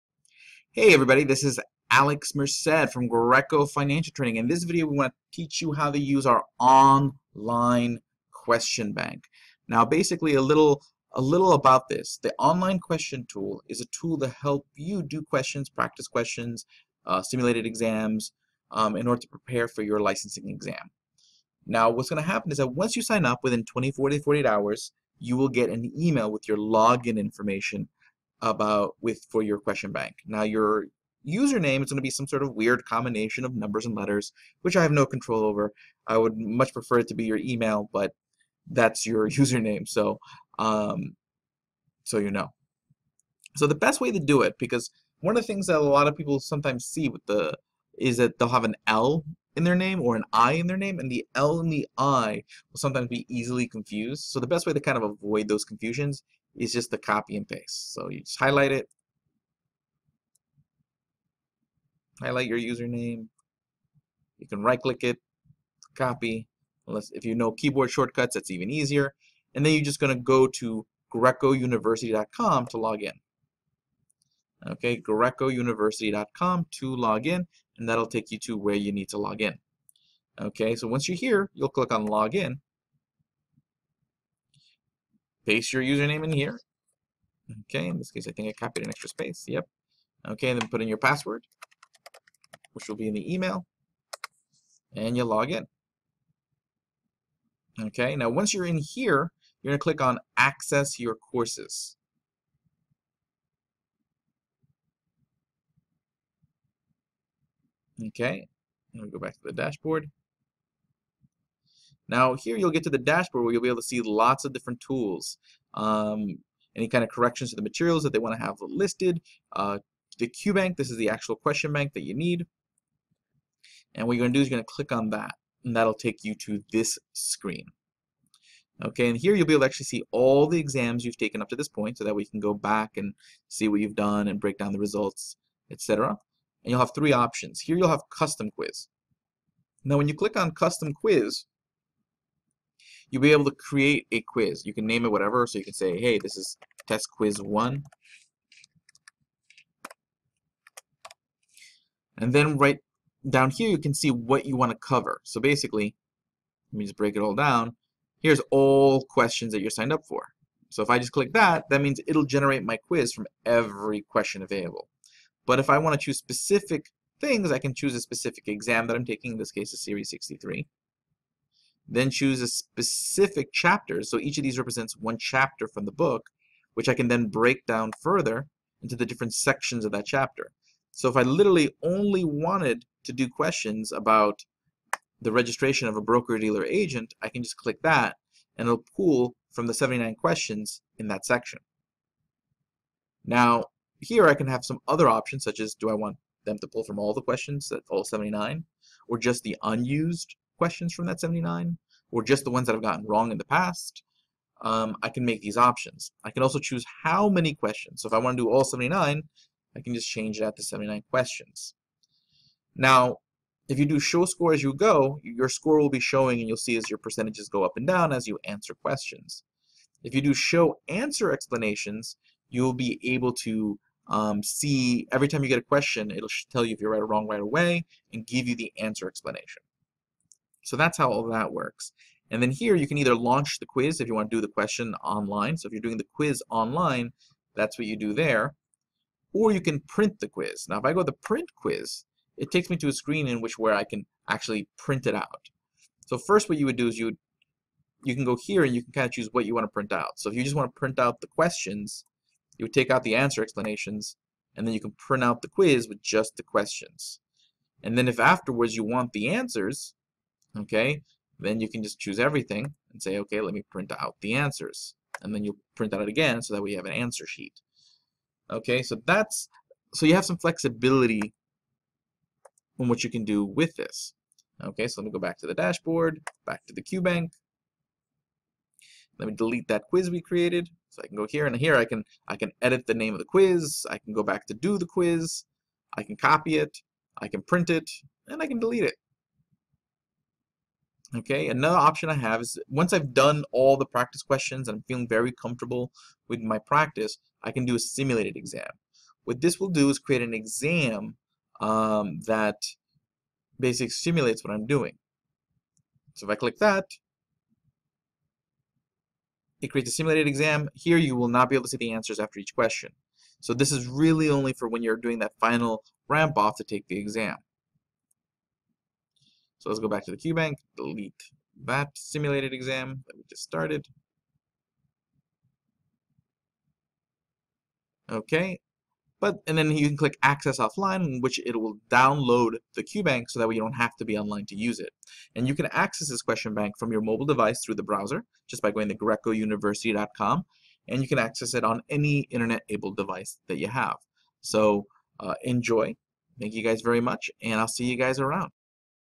How to use the GreicoUniversity.com Question Bank - Greico Financial Training (Greico.com)
Alex Merced goes through how to use the Greico Online Question Bank for preparing for exams like the SIE and Series 7.

Purchase an Online Question Bank at Greico.com